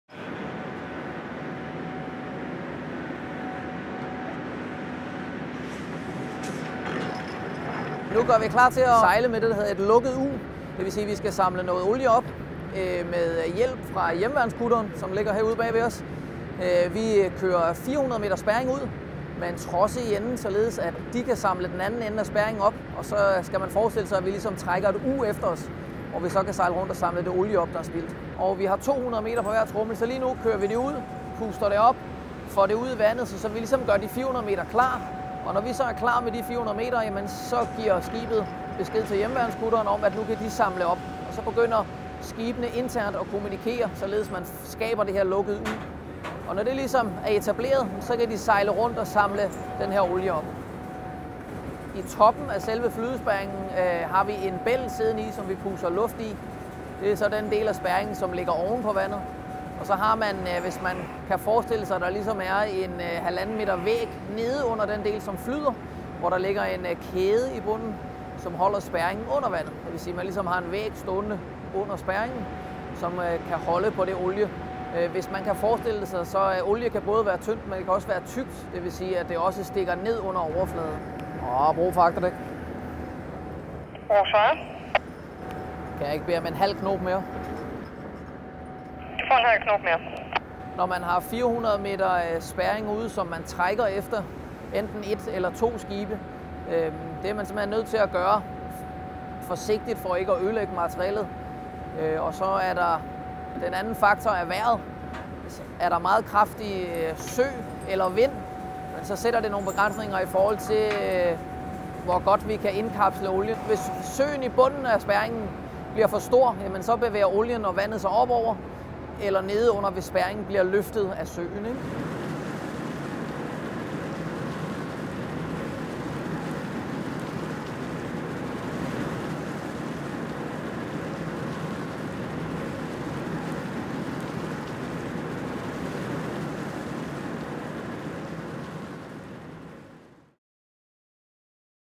Hvordan indsamler Forsvaret olie fra olieudslip?
Forsvaret har en række miljøskibe, som mestrer opgaven. Se her, hvordan man gør.